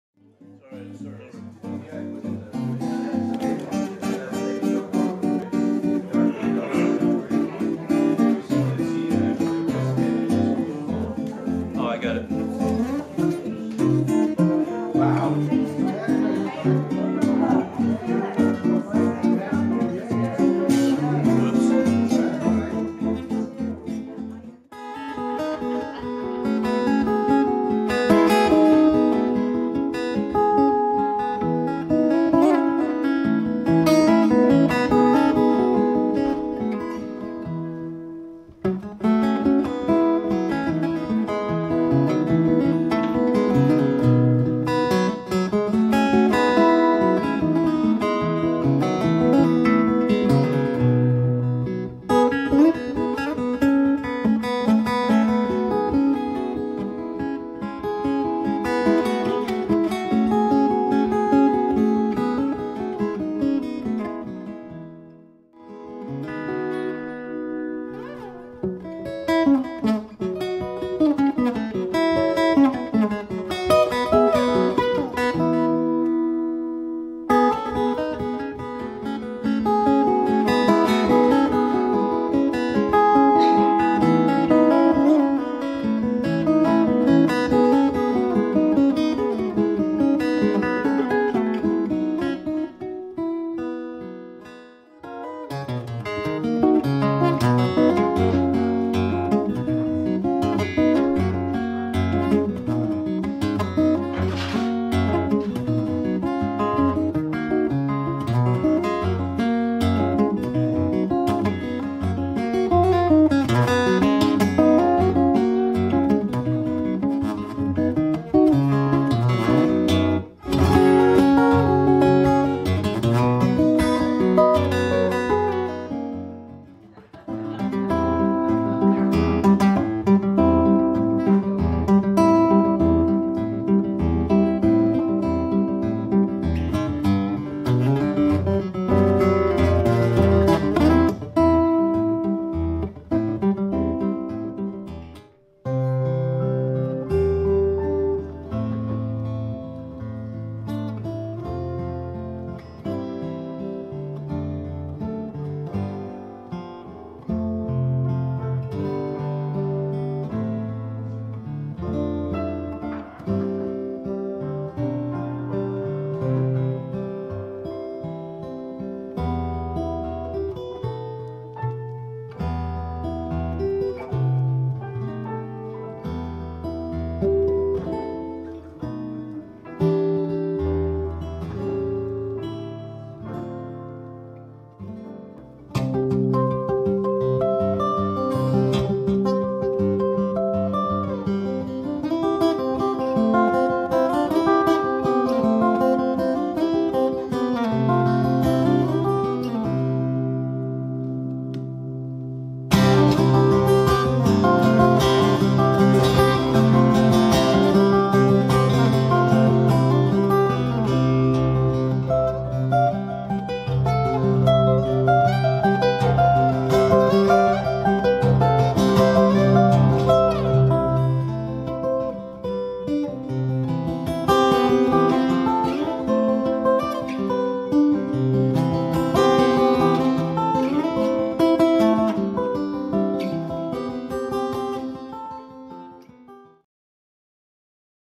Richard Gilewitz in Jupiter
Guitar virtuoso Richard Gilewitz performed recently in Jupiter.  Here is a sample of the work.  Full video coming soon.  Enjoy and share.